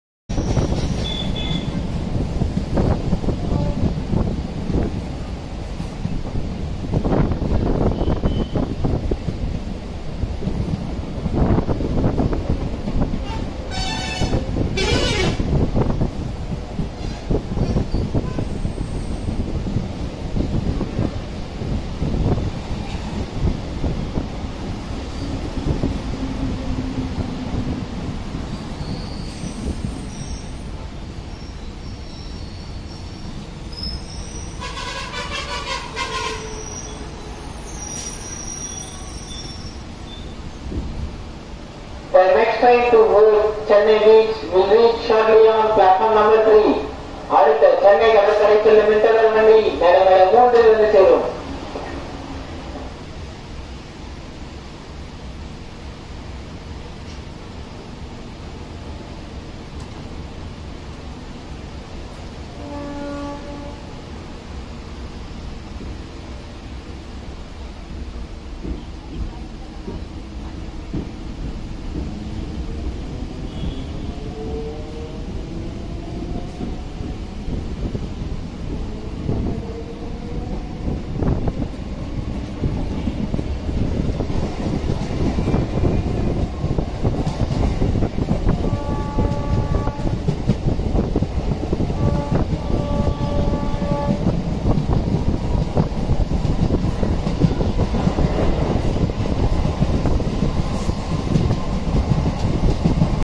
Potheri | POTI | पोत्तेरी | பொத்தேரி | Kattankulathur | SR | Chennai
Potheri railway station is one of the railway stations of the Chennai Beach–Chengalpattu section of the Chennai Suburban Railway Network. It is in between the neighbourhood of Guduvancheri and Kattankulathur in Chennai.

Potheri is a rapidly-growing village locality in the South suburbs of the Metropolitan city Chennai. It is a town that comes under Maraimalai Nagar municipality of Chengalpattu taluk in the Chengalpattu district of Tamil Nadu, India and suburb of Chennai within Chennai Metropolitan Area.

It is situated at a distance of 43 km (27 mi) from Chennai Beach junction and is located on NH 45 in Potheri, with an elevation of 45 m (148 ft) above sea level.